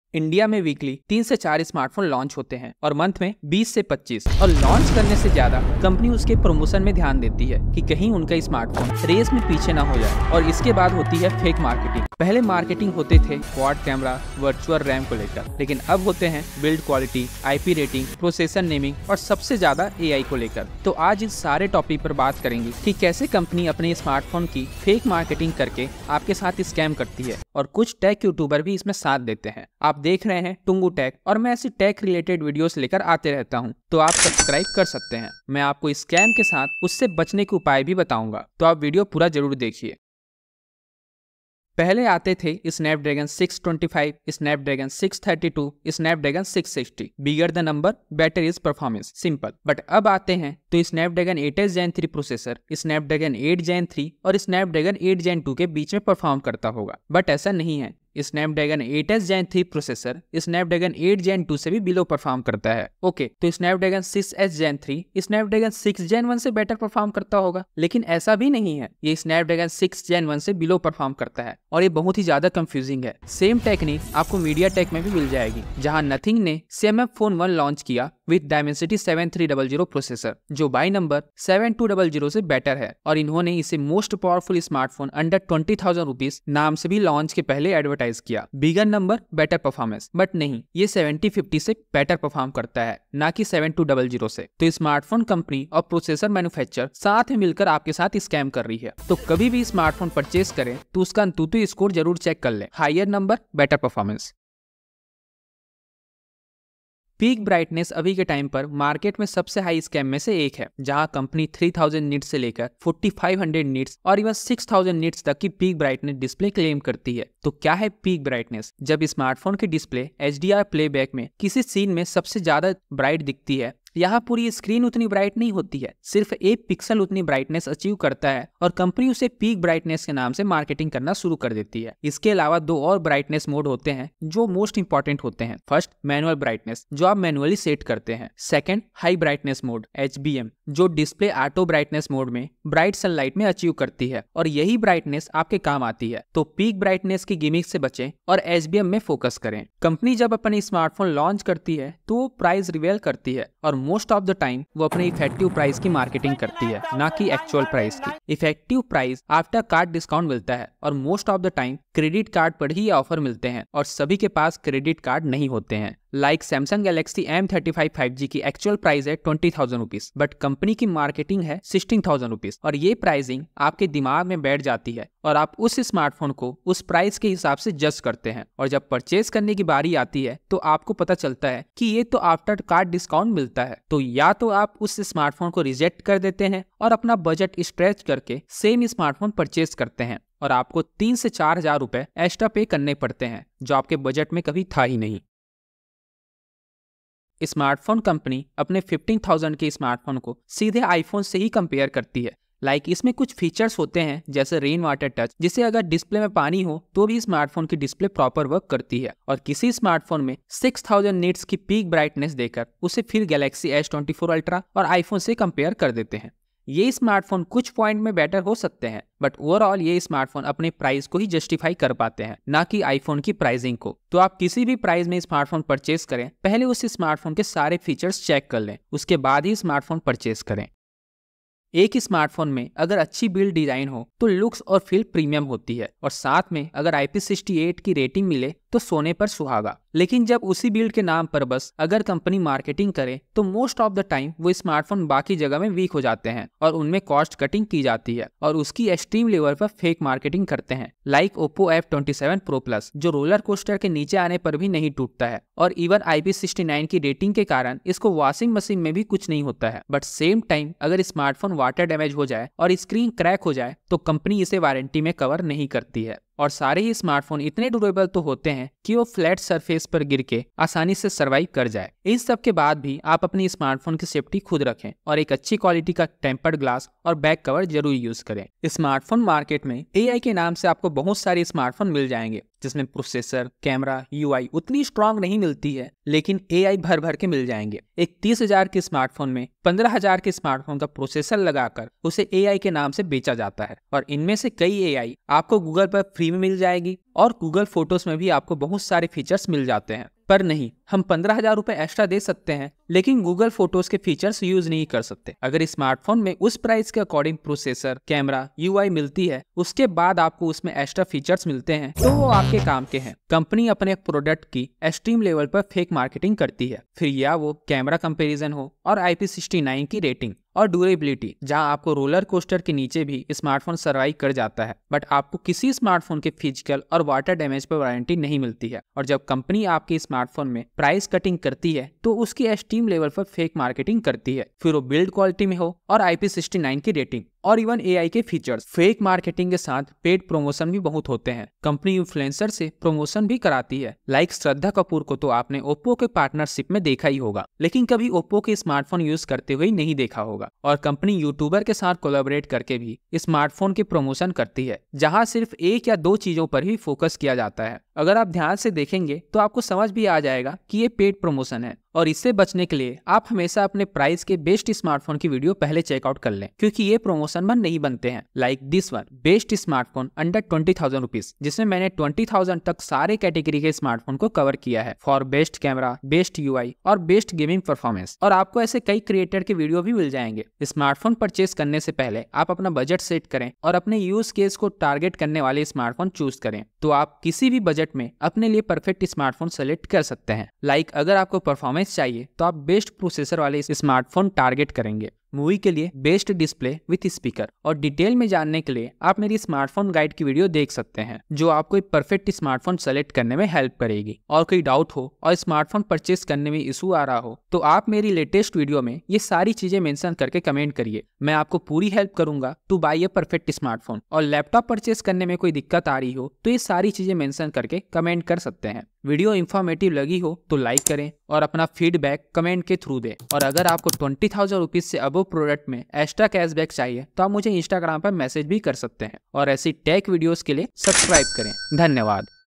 Don't Trust Smartphone Ads ! ye hai sachchai
Aapko kyu lagta hai ki jo cheezein company aapko smartphone ke ads mein dikhati hai, vo aapke phone mein waise hi kaam karti hai? Aur jo extra features ke bare mein bataya jata hai, vo sab milte hain? Aisa nahi hota! Company apne product ki extreme level par fake marketing karti hai, aur inhi sab marketing tactics ko maine is video mein cover kiya hai. Toh aap video ko pura dekhein!

नोट: वीडियो में दिखाए गए सभी चित्र/फोटो उनके संबंधित मालिकों के हैं। वीडियो में प्रदर्शित किसी भी चित्र का स्वामी मैं नहीं हूं।

Time codes

0:00 - Introduction
0:41 - processor naming scam
1:45 -  Peak Brightness
2:30 - Effective Price
3:15 -weird comparision
3:51 -design and build
4:39 - AI marketing
5:14 - Fake marketing
6:02 conclusion